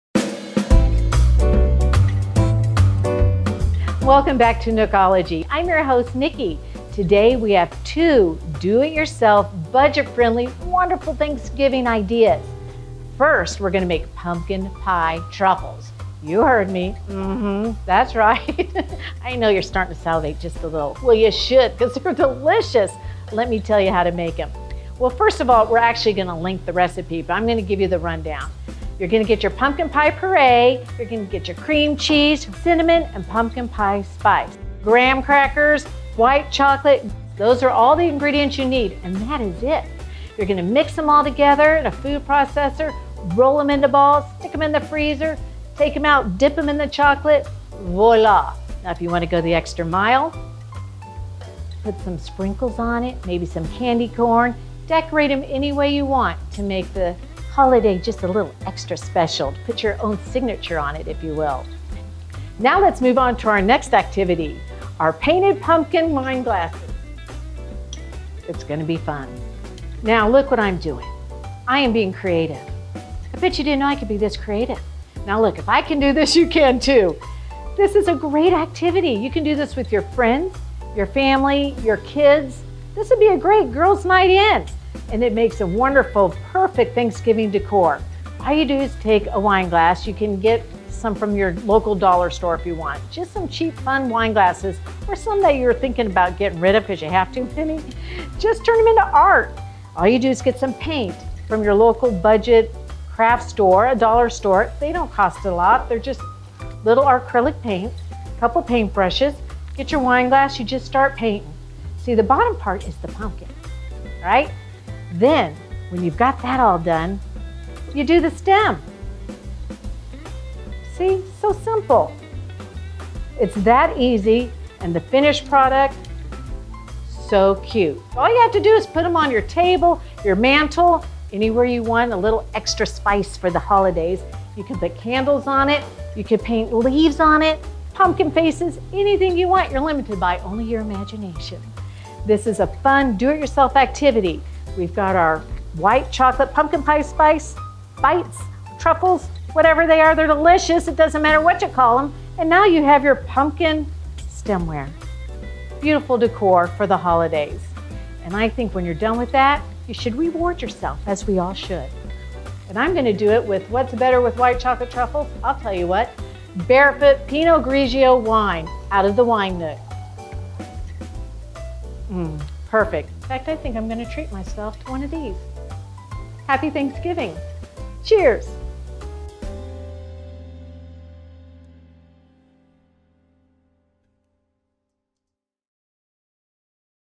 Nookology--Thanksgiving activities
Learn to make delicious pumpkin spice truffles and a turn a wine glass into a cute Thanksgiving decoration